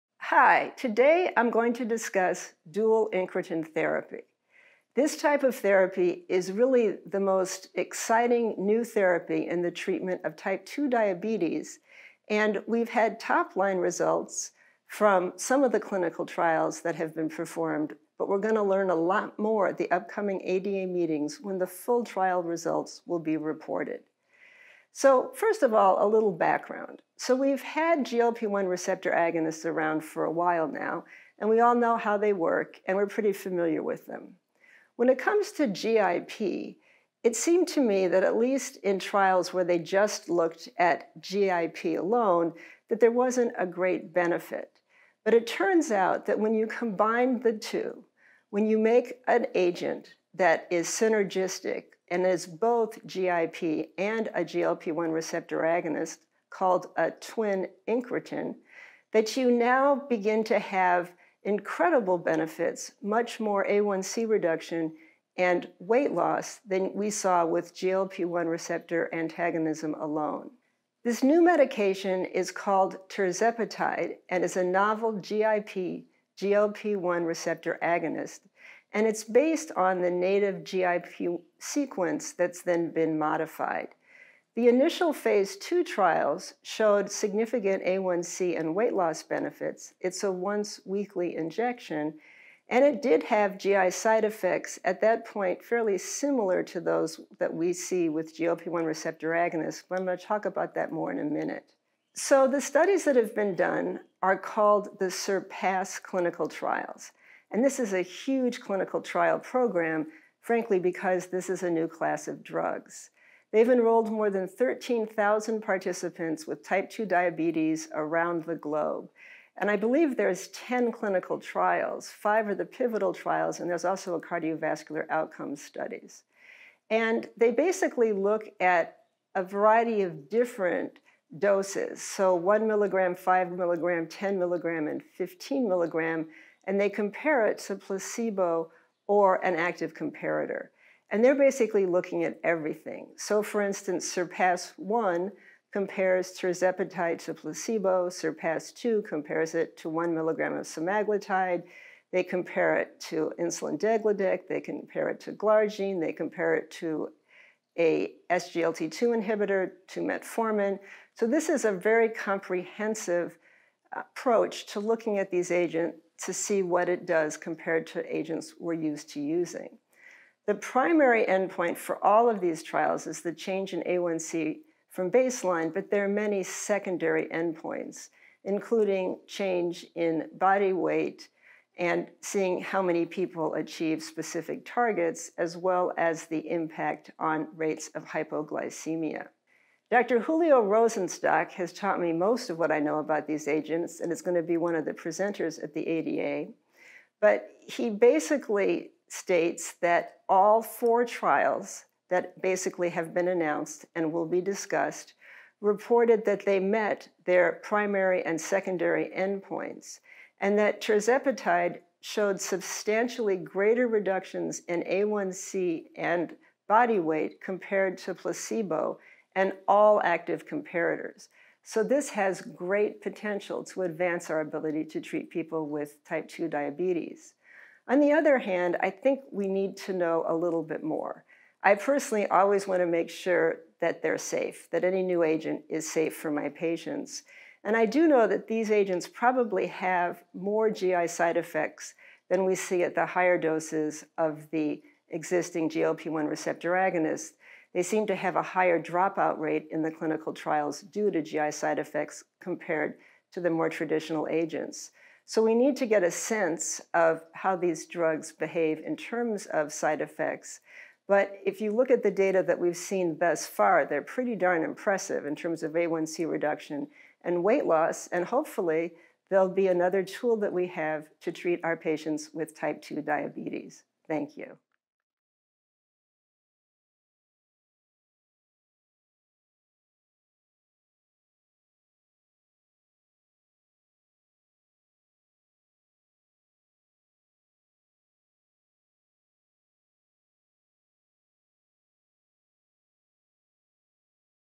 Peters on Tirzepatide: 'Impressive, but We Need to Know More'
Dr Anne Peters discusses tirzepatide and the topline results from the SURPASS trials.